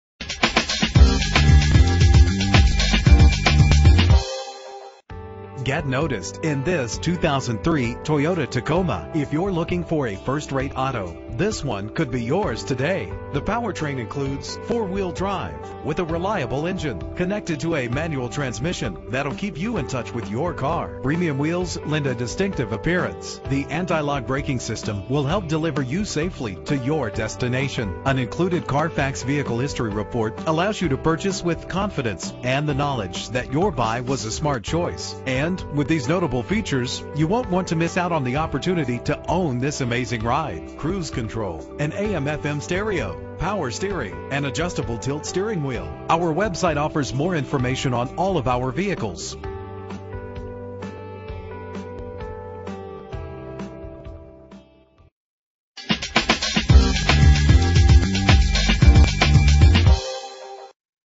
2003 Toyota Tacoma - Brookpark OH
Year: 2003
Make: Toyota
Model: Tacoma
Trim:
Engine: 2.7 L, 4 cylinder
Transmission:
Color: Gold
Mileage: 89224
Address:
13775 Brookpark Road